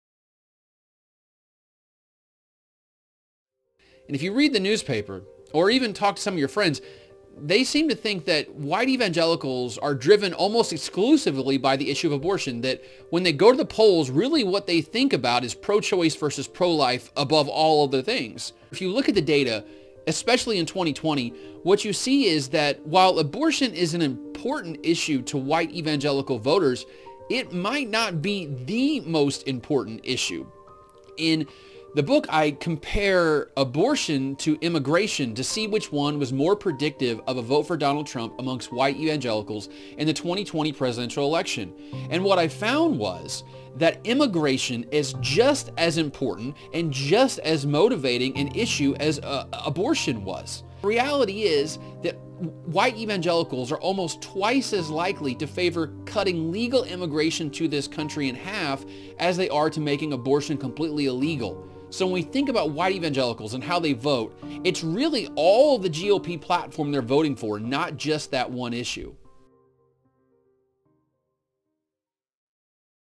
Author Ryan Burge on Myth #11: Abortion is the most important issue for evangelical voters.
Author Ryan Burge discusses Myth #11 from his new book, "20 Myths about Religion and Politics in America"